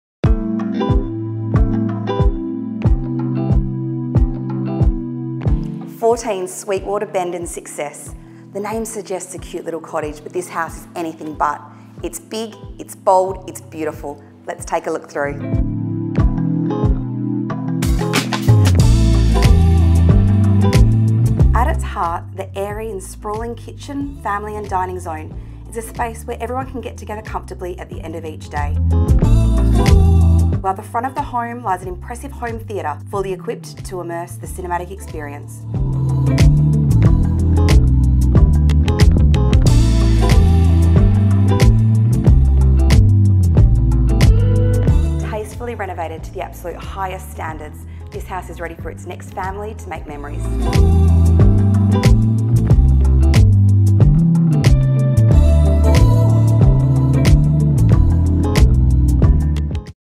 Kirstin Dunn and Semple Property Group are proud to present 14 Sweetwater Bend, Success.
How Sweet it is….

The time has come for a new family to create happy memories in this stunning contemporary home exuding spacious, light-filled and modern interiors. Showcasing LED lighting, hard wearing wood look tiled flooring and flawless finishes throughout, 14 Sweetwater Bend is waiting for you now in the popular suburb of Success.

At its heart, the airy, sprawling kitchen/family/dining zone is a space where everyone can gather together comfortably at the end of each day or to celebrate on a larger scale. While an impressive home theatre like no other is fully equipped to offer an immersive cinematic experience. Welcome to a home that effortlessly offers options to entertain or relax in style.

Features and Inclusions:
• Spacious entry hall with large storage cupboard
• Theatre room with full projector, huge screen, plush carpets, bifold doors plus walls and ceiling painted a dark hue
• Open plan family and meals PLUS separate games room, offering plenty of space to host large gatherings
• Stunning gourmet kitchen with quality granite bench tops, ample storage options, feature LED lit granite splash back, pantry, stainless steel appliances, double fridge recess with plumbing point, gas cooking, plus built in electric oven and microwave
• King size main bedroom with built in wardrobes, stunning feature pendant lighting
• Luxurious private ensuite with dual shower, twin basin vanity and separate toilet
• Generous minor bedrooms with built in wardrobes
• Home office or kids activity area
• Fully renovated family bathroom with vanity, shower, toilet
• Renovated laundry with feature splash back, plenty of bench space and storage cupboards
• Ducted reverse cycle air conditioning throughout
• Security alarm and camera system including camera door bell
• Solar panels
• Paved alfresco entertaining area
• Built-in vacuum
• Built-in speaker system
• Low maintenance bore reticulated lawns and gardens
• Side access
• Double lock up garage with wifi operation to access plus shoppers entrance directly into the home
• Large garden shed

Property Facts:
• Council Rates: $1,758 approx. pa
• Water Rates: $1,422.66 approx. pa
• Block: 600sqm
• Living: 220sqm
• Built: 2007

Nearby amenities include:
• Success Primary School – 900m
• The Hive Park Shopping Centre & IGA – 1.8km
• Cockburn Gateway Shopping Centre – 2.7km
• Cockburn Train Station – 3.3km
• Fiona Stanley Hospital – 11.1km